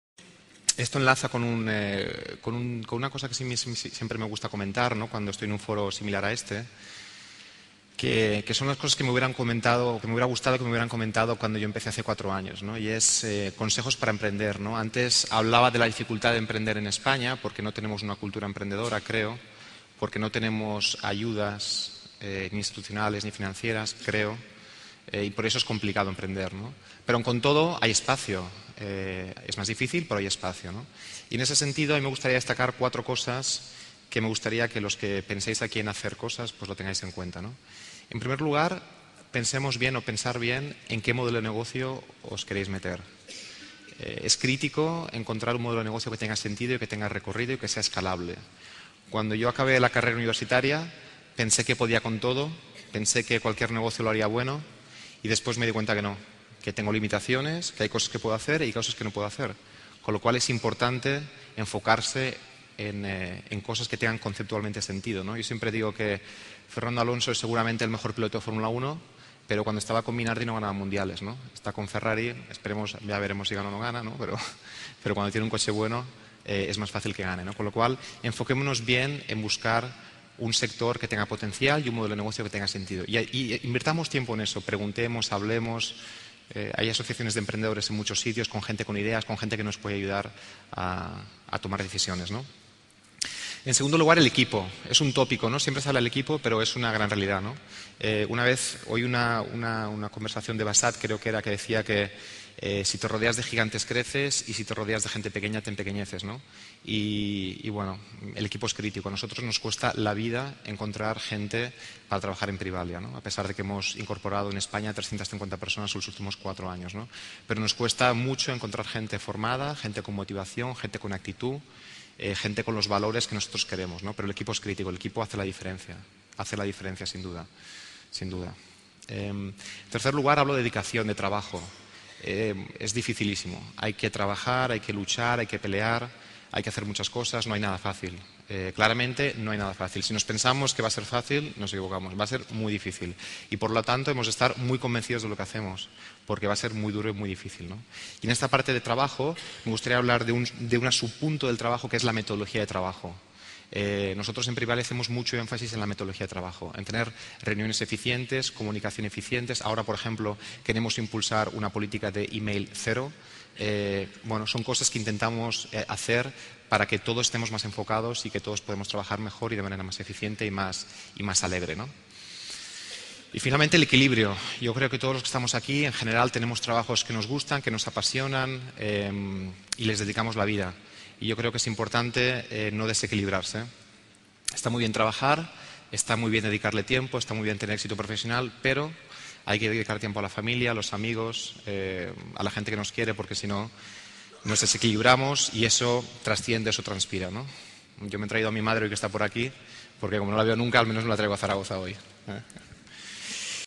Foro Aragón Empresa 2010 José Manuel Villanueva de Privalia 1/2
José Manuel Villanueva socio fundador de PRIVALIA en el Foro ARAGÓN EMPRESA 2010  1/2